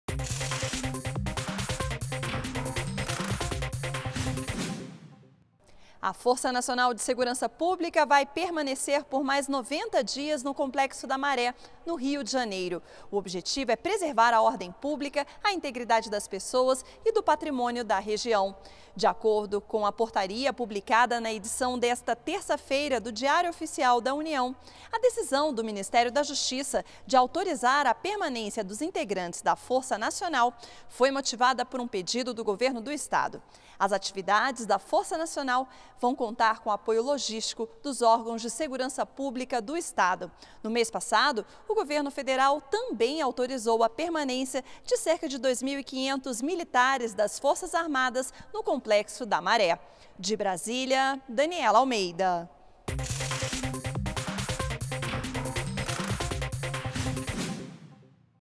Força Nacional de Segurança vai permanecer por mais 90 dias no Complexo da Maré
ÚLTIMAS - 12.08.14: Autorizada a permanência da Força Nacional de Segurança Pública no Complexo da Maré, no Rio de Janeiro, por mais 90 dias. A medida, publicada no Diário Oficial da União desta terça-feira (12), visa preservar a ordem pública, a integridade das pessoas e do patrimônio da região.